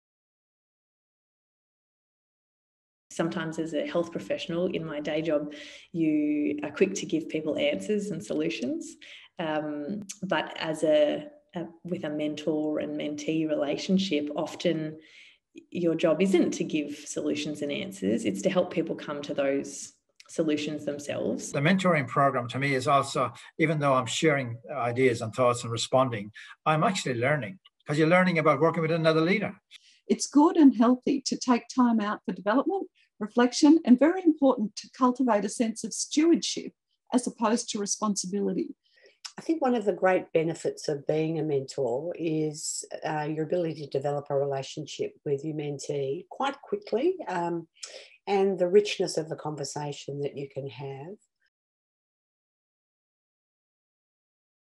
Tri-Peaks mentoring program
In this video we hear from mentors and mentees that took part in the Tri-Peaks mentoring program.

The Tri-Peaks project is a collaboration between The Centre for Excellence in Child and Family Welfare, the Victorian Healthcare Association and the Victorian Alcohol and Drug Association – funded by the Departments of Health and Families, Fairness and Housing.